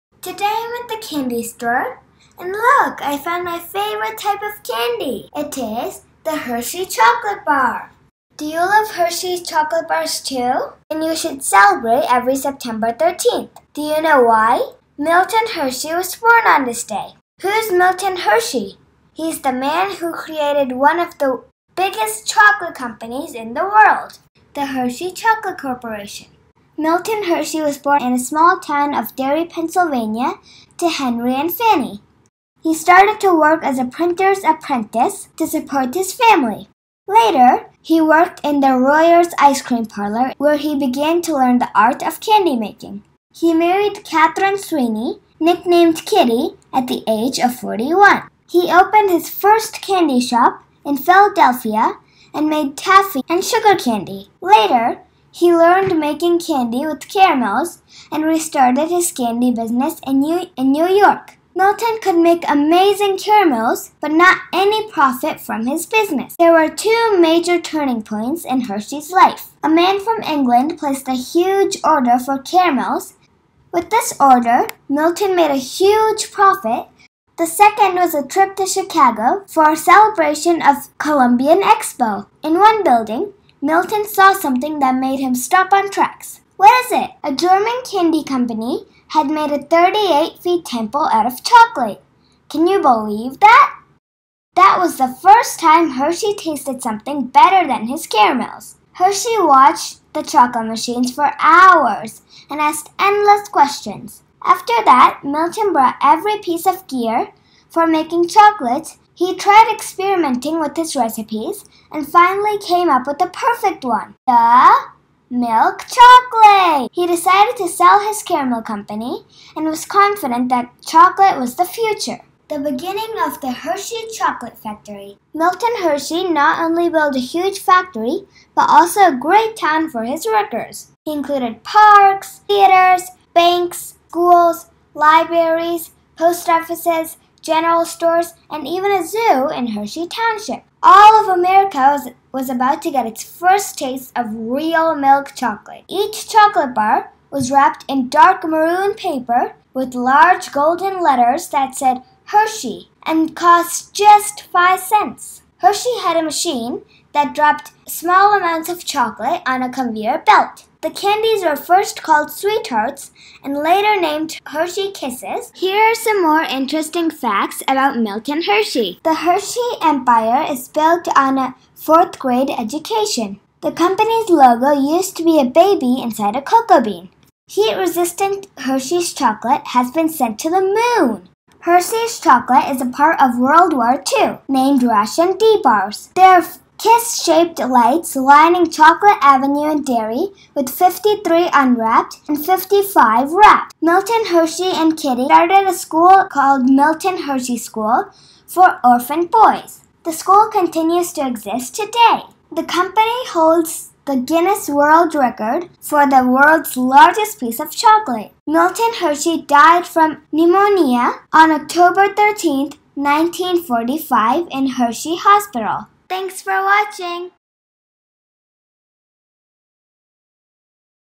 Biography of Milton Hershey for kids/ The Hershey's Chocolate Factory
Do you love  Hershey chocolates ?Did you know that someone called Milton  Hershey made it!!! learn more in this video